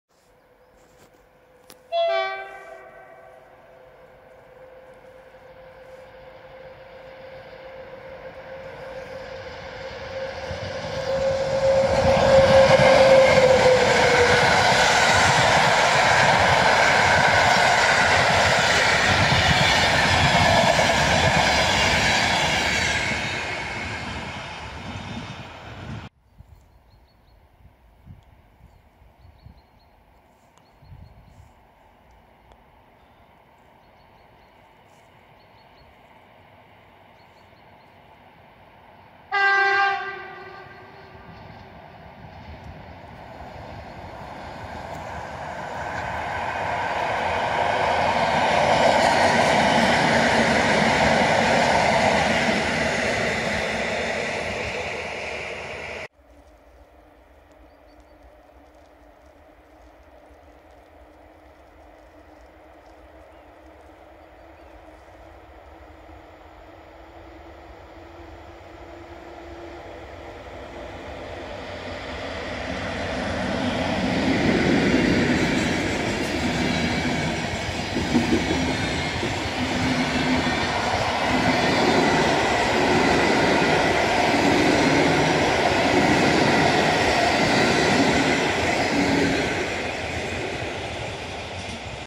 Trzy pociągu na nieczynnym już posterunku odgałęźnym Magdalenka. 19.03.2023
|Magdalenka| 3 pociągi na nieczynnym posterunku podg. Magdalenka:
0:00 EP07-388 z |Za Flirt| IC 11151 "Kraszewski" Warszawa Wschodnia- Terespol.
0:26 EN57ALd-2227 jako R 12204 Terespol- Lublin Główny.
0:56 EP07-388 z |Za Flirt| IC 11152 "Kraszewski" Terespol- Warszawa Centralna.

Pozdrawiam.